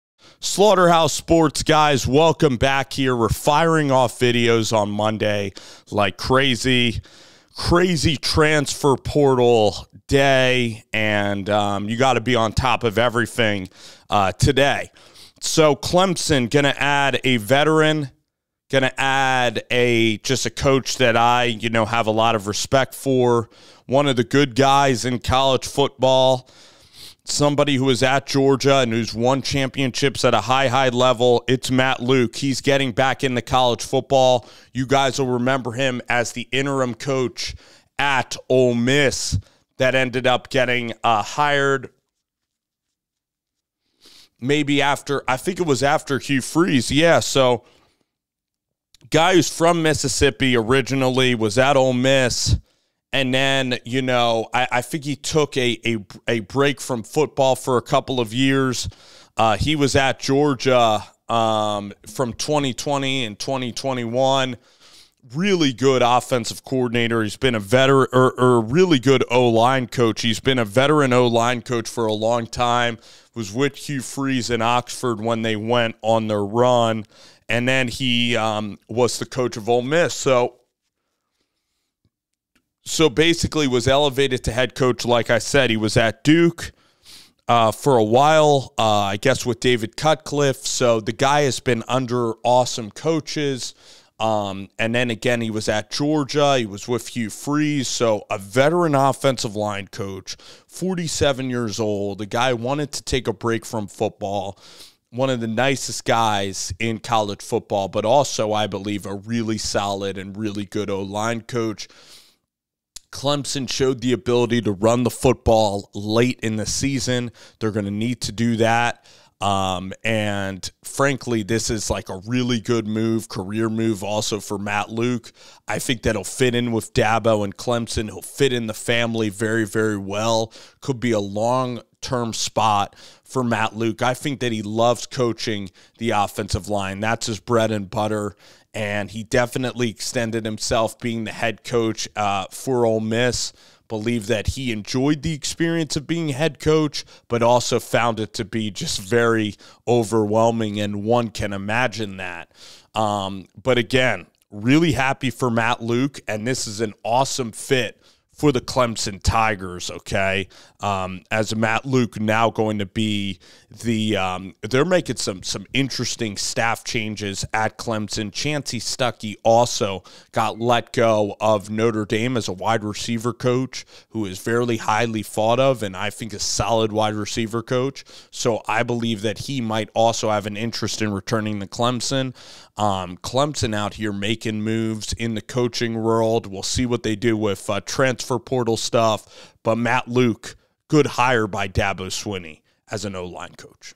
Matt Luke Hired at Clemson as OL Coach
Matt Luke a vet. Former Georgia o line coach and longtime coach for Ole Miss. Was a head coach at Ole Miss after Hugh Freeze. Very good o line coach, great guy. I like the hire for Clemson and dabo